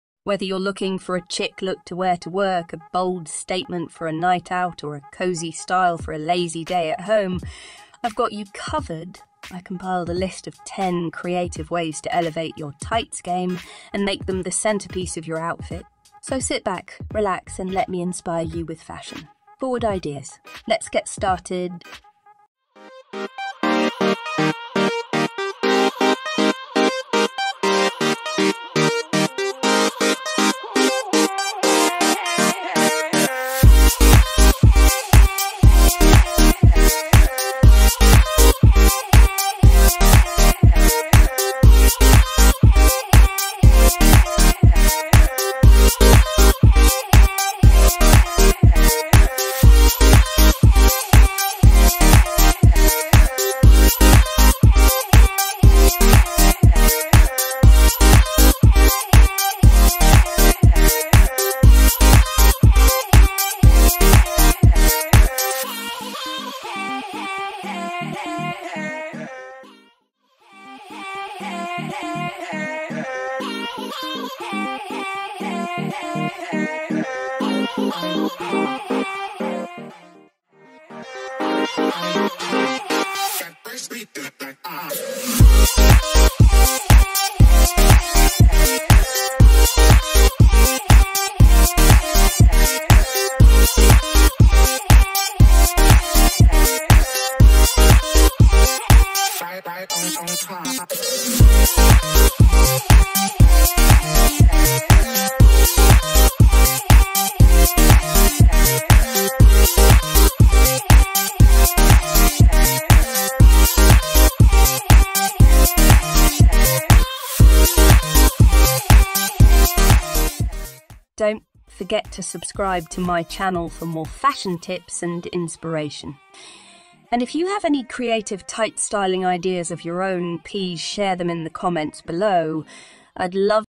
TIGHTS LOOKBOOK | 10 Creative Ways to Style Your Tights
In this video, our team of experts has compiled a list of 10 creative ways to take your tights game to the next level. Whether you're dressing up for a night out or going for a casual look, we've got you covered with fashion-forward ideas that are sure to impress.

We cover everything from classic tights styling to more unconventional ways to wear them, so you're sure to find something that suits your style. Plus, we'll give you some tips on how to accessorize and pair your tights with other wardrobe staples to create a complete and stylish outfit.

So, if you're looking to add some excitement to your tights game and stand out from the crowd, be sure to watch this video and try out some of our creative ideas.